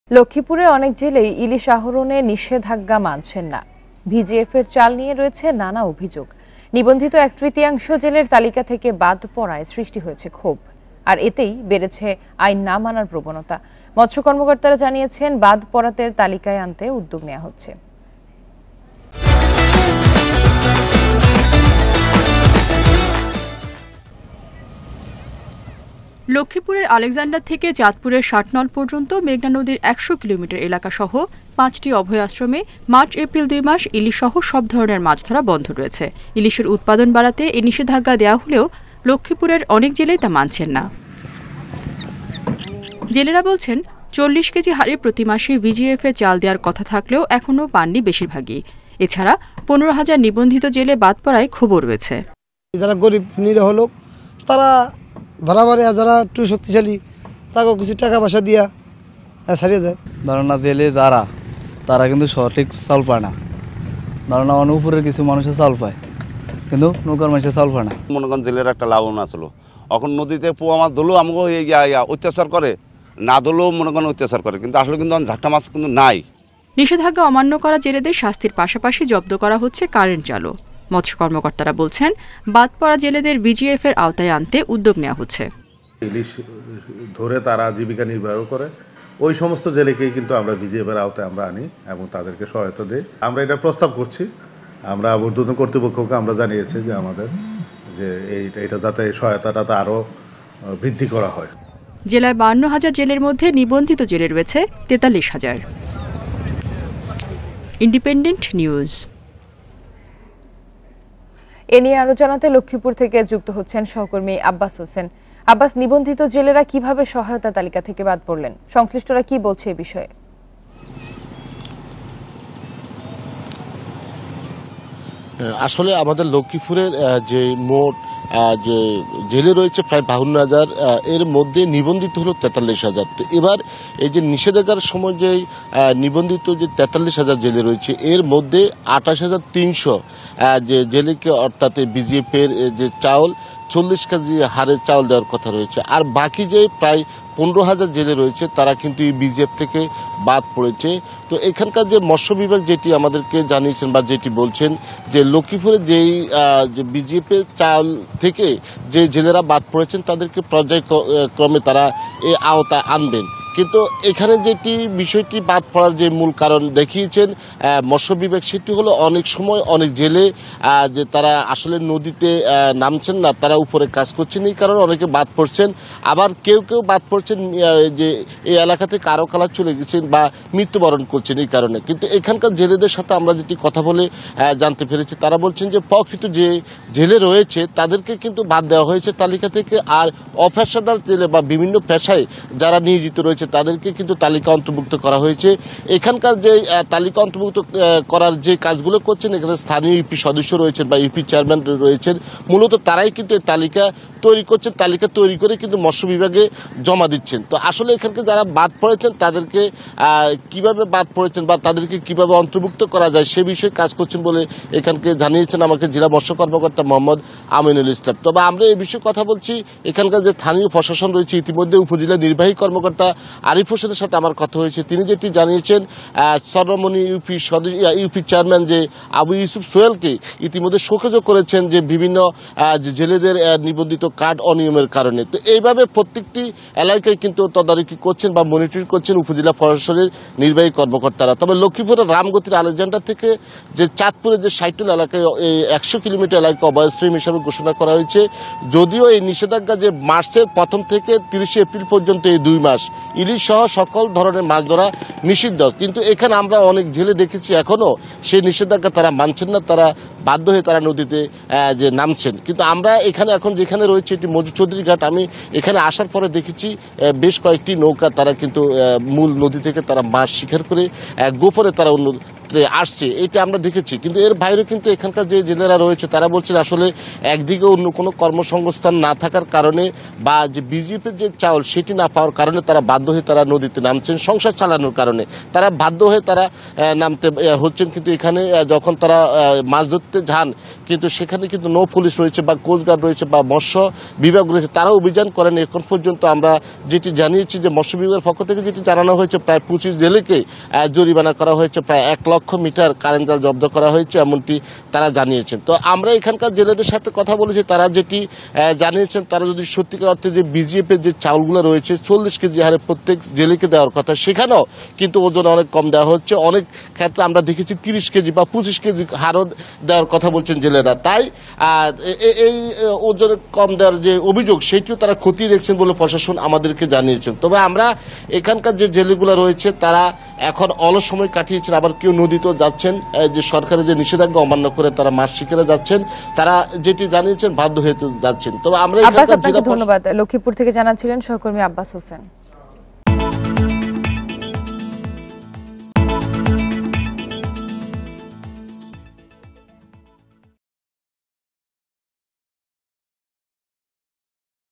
জেলেদের শাস্তির পাশাপাশি জব্দ করা হচ্ছে কারেন্ট জালও | Ban of Hilsa Fishing | Independent TV
লক্ষ্মীপুরে অনেক জেলেই ইলিশ আহরণে নিষেধাজ্ঞা মানছেন না। ভিজিএফের চাল নিয়ে রয়েছে নানা অভিযোগ। নিবন্ধিত এক-তৃতীয়াংশ জেলের তালিকা থেকে বাদ পড়ায় সৃষ্টি হয়েছে ক্ষোভ। আর এতেই বেড়েছে আইন না মানার প্রবণতা। মৎস্য কর্মকর্তারা জানিয়েছেন, বাদ পড়াদের তালিকায় আনতে উদ্যোগ নেয়া হচ্ছে।

Many fishermen in Lakshmipur do not obey the ban on hilsa extraction. There are many complaints about VGF rice. One-third of registered jails were left off the list, sparking outrage. And this has increased the trend of disobeying the law. Fishery officials said that initiatives are being taken to bring those excluded to the list.